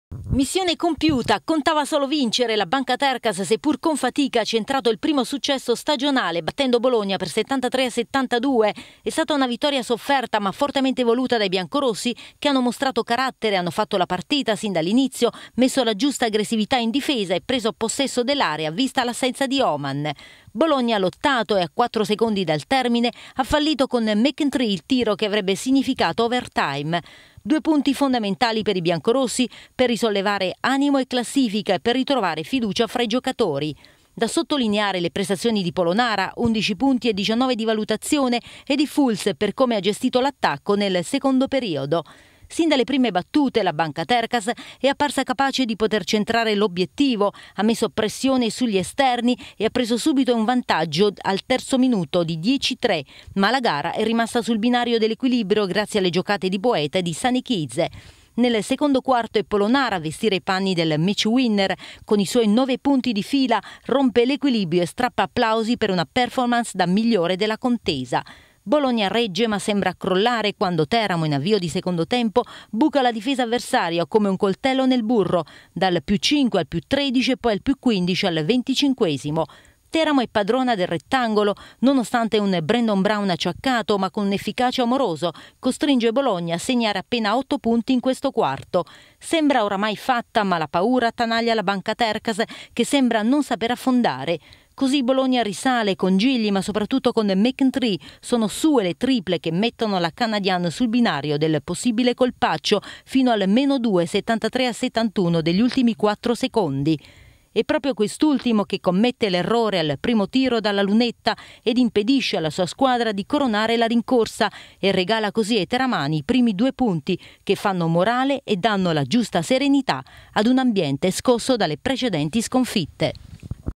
BASKET: BANCATERCAS- CANADIAN SOLAR BOLOGNA
Prima vittoria in campionato per la Bancatercas Teramo contro la Canadian Solar Bologna (73-72)